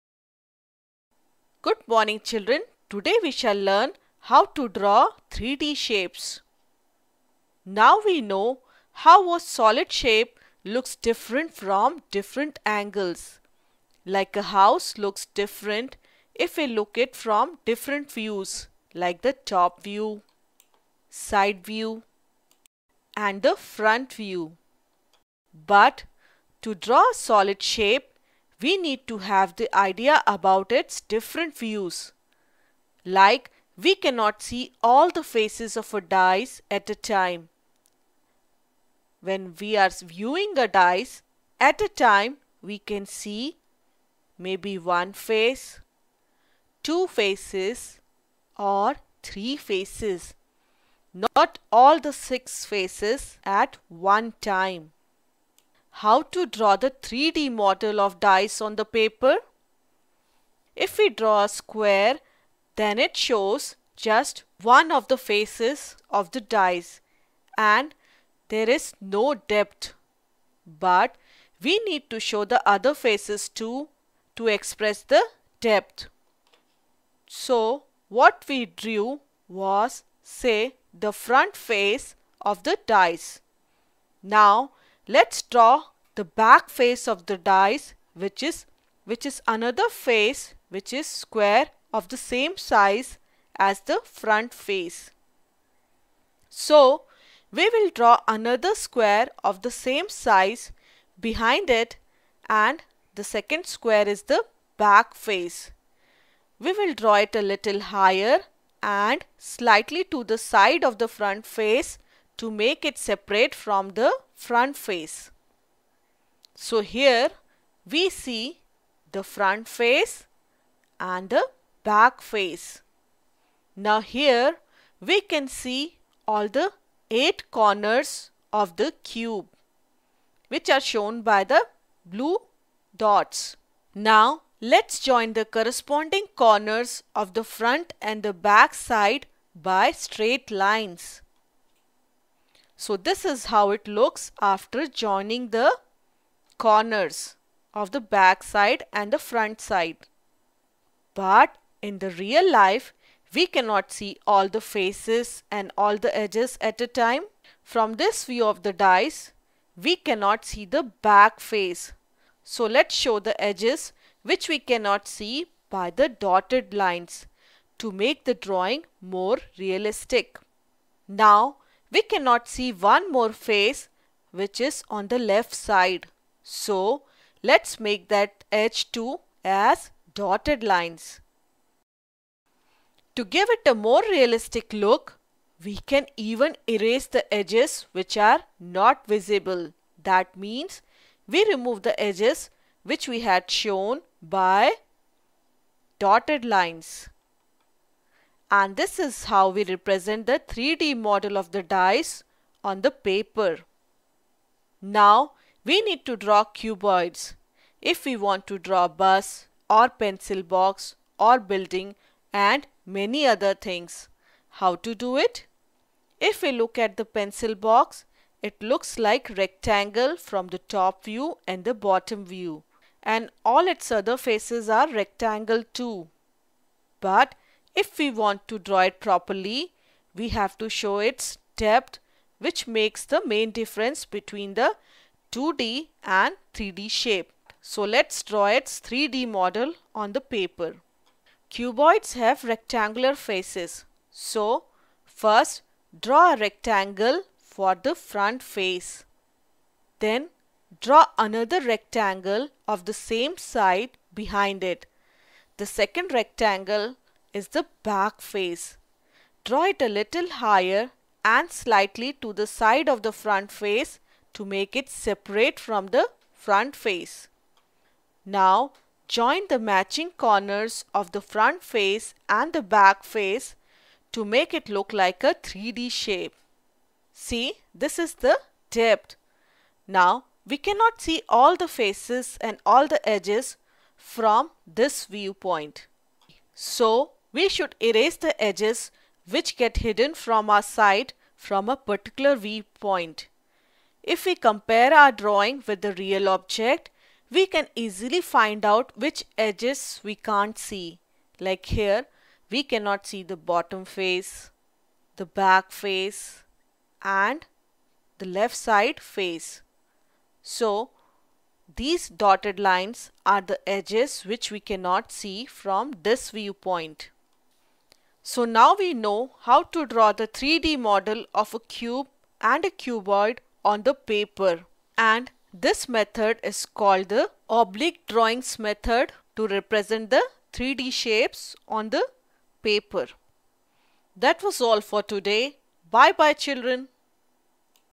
Math - How to draw 3D or solid shapes (cube, cuboids) on paper - English
Hello,
BodhaGuru Learning proudly presents an animated video in English which describes how to draw 3D objects like cube, cuboids etc expressing its depth and how drawing of a solid shape from different view point differs.

Team BodhaGuru